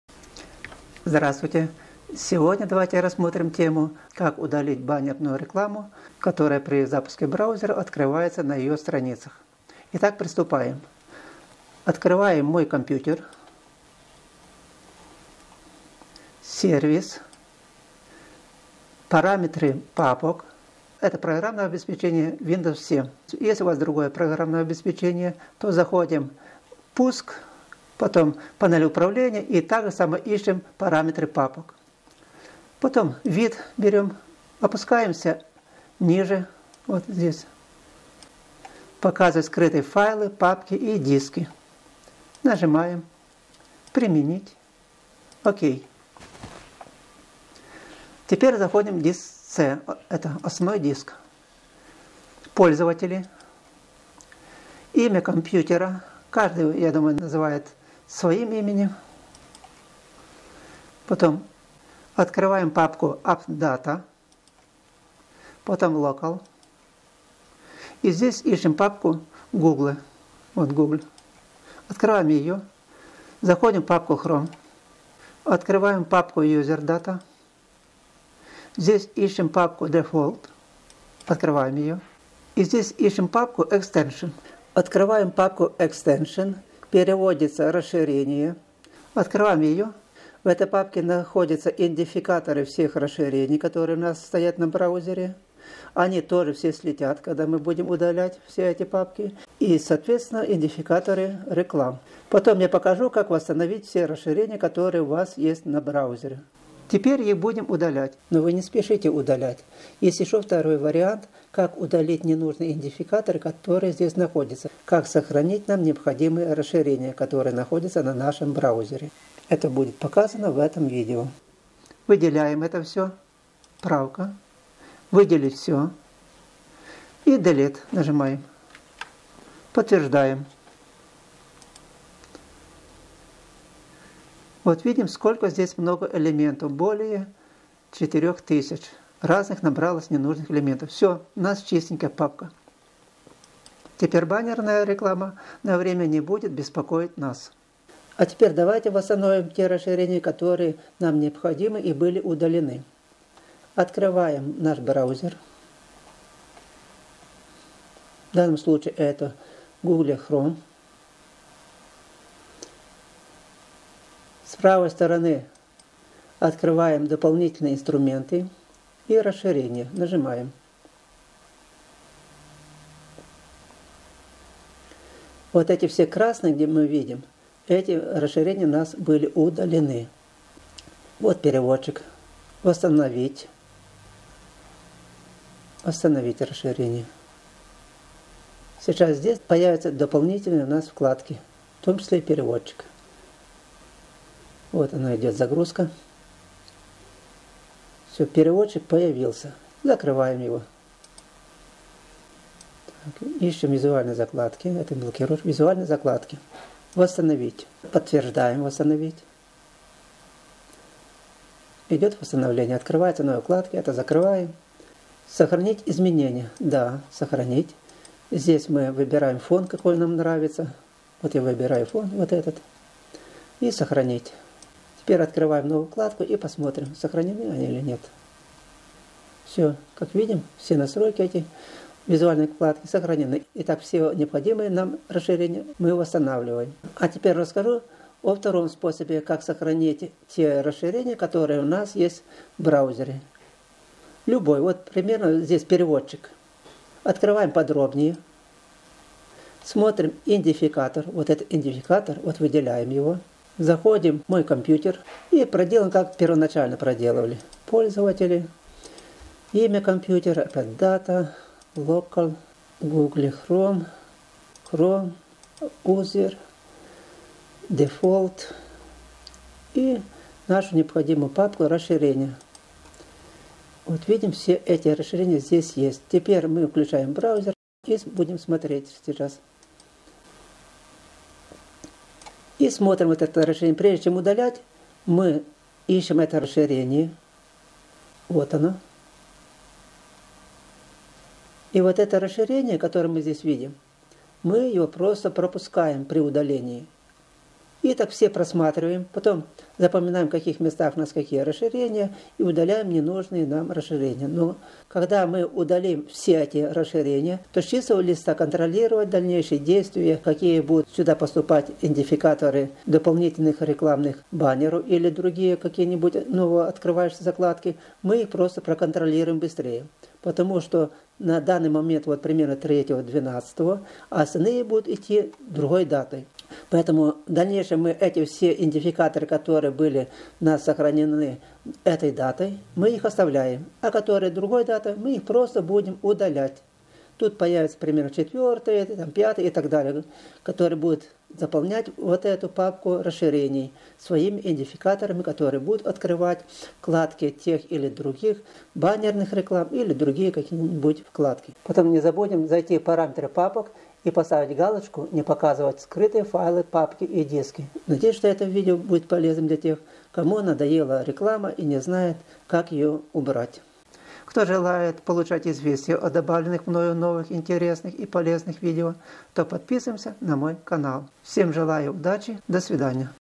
Как убрать всплывающую рекламу в браузере google chrome .Удаление всплывающей рекламы
Как убрать всплывающую рекламу в браузере без  программы.Удаление всплывающей рекламы в браузере. Удаление всплывающих окон рекламы без вреда программному обеспечению вручную использую основы интернета для начинающих.Делаем с осторожностью не спешим,что бы случайно не удалить необходимое обеспечение для работы браузера в интернете.Есть несколько способов как убрать всплывающую рекламу в браузере google chrome -это вручную и с помощью программ,которые блокируют её.Здесь мы разберём один из них-это удаление расширений идентификатора баннеров рекламы,которые всплывают при запуске браузера.Удаление всплывающих окон рекламы,которая так часто появляется на страницах просмотра порой не увенчивается успехом.Эту работу необходимо проделывать регулярно,хотя бы один раз в 7-10 дней.При открытии любых страниц из интернета навязчивая реклама самостоятельно без нашего ведома вписывает свой идентификатор в расширения браузера,которые потом в виде всплывающих окон рекламируют свой товар.Часто мы задумываемся как удалить всплывающую рекламу в браузере и не всегда получается добиться своего.Полностью от этого мы не избавимся.Есть один выход-не посещать страницы сайтов в интернете.Но это не то.Будем просматривать необходимые или интересующие нас сайты или другие источники,но после этого будем помнить,что идентификаторы вползающих баннеров рекламы всегда будут преследовать нас.Частично можно избавиться от этого,но не полностью.Для этого необходимо применить программы запрещающие появляться всплывающим окнам баннерной рекламы.Но это будет в следующем видео.А пока смотрим как убрать всплывающую рекламу в браузере  google chrome без программы и проделываем необходимые меры удалением их из компьютера.Удаление всплывающей рекламы,которая  на время не будет беспокоить нас.